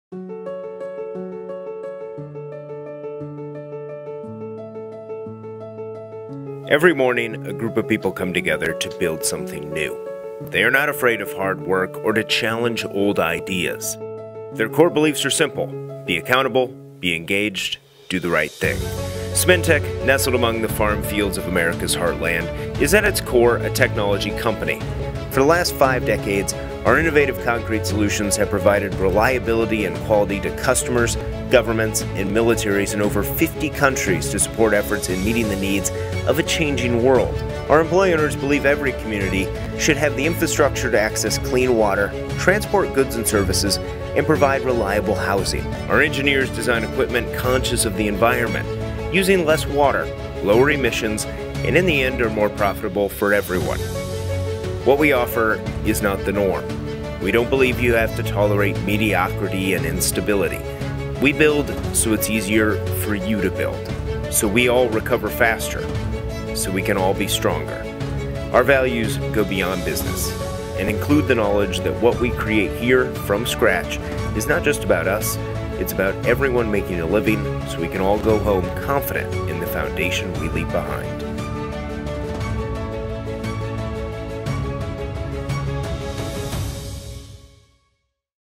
Why We Build Volumetric Mixers
Cemen Tech mobile and stationary concrete mixers are used in over 60 countries and have provided the most advanced concrete productions solutions to customers for almost five decades.